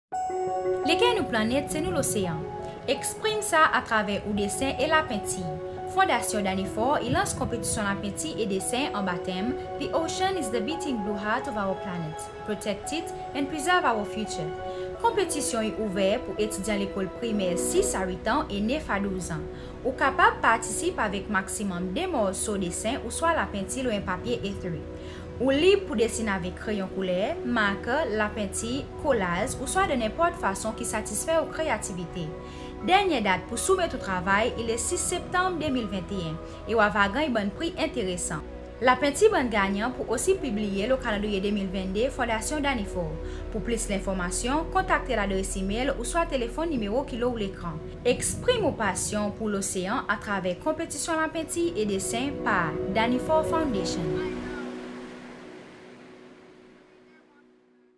DANNY FAURE FOUNDATION'S PAINTING COMPETITION
The theme is The ocean is the beating blue heart of our planet. Protect it and preserve our future.
The drawing competition is open for children from 6-8 and 9-12 years old.
Each child can submit 2 artworks. The style is free (coloured, pencils, crayons, collage, markers etc...)
Deadline to submit is 6 September 2021.
The 12 winning paintings will feature on the Danny Faure Foundation's 2022 calendar.